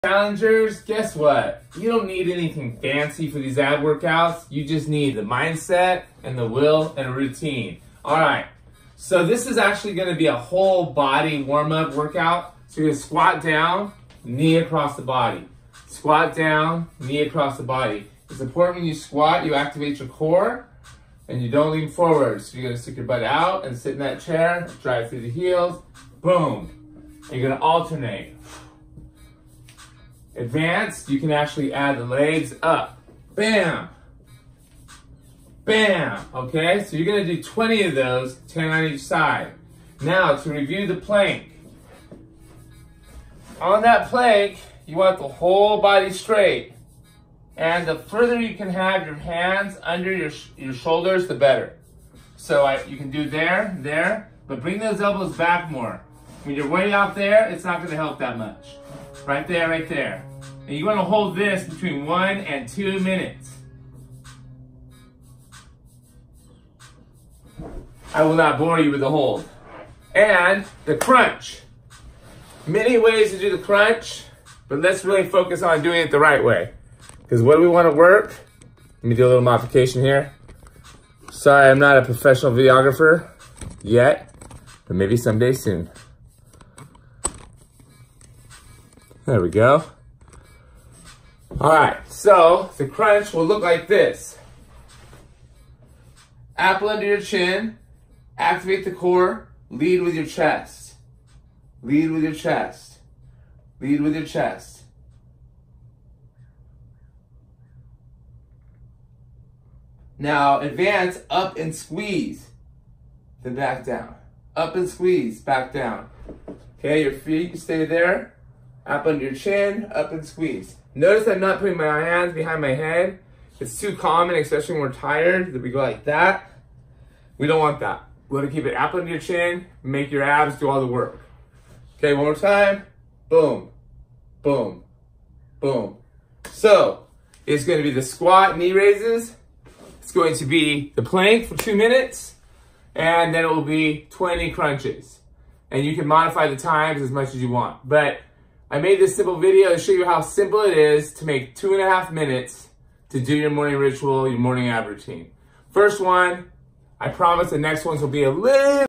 Squat Knees plank crunches
The three basic movements for the Belly Blast Morning Ritual.